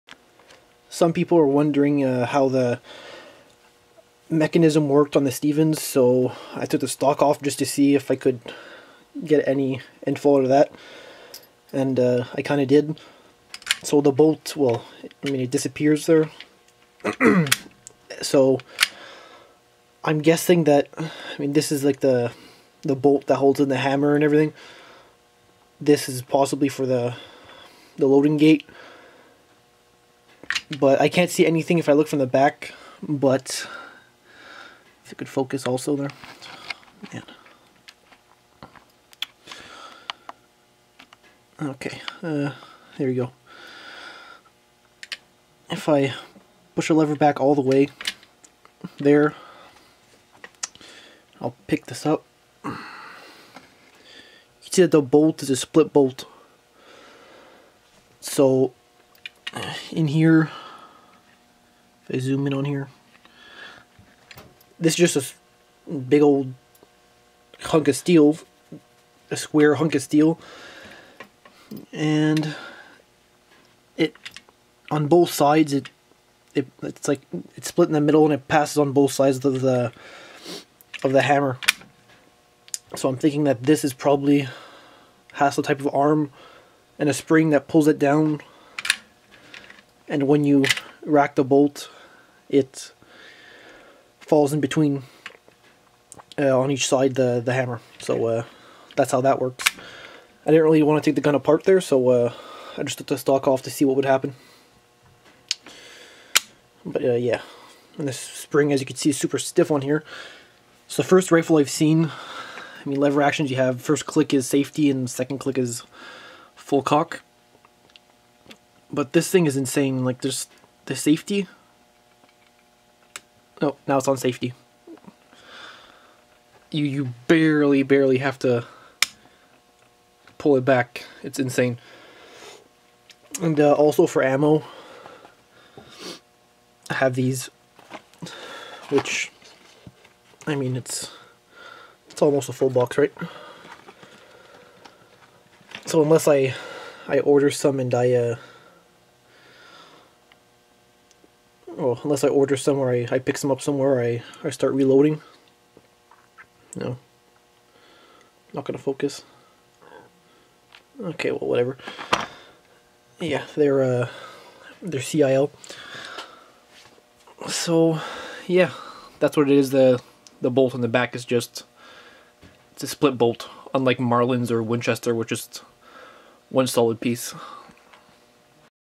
Stevens High Power Model 425 Mechanism.
Its not an in -depth disassembly video or anything but long story short: The bolt has a pivot that makes it travel diagonally. It also has a split down the middle on the rear to make space for the hammer.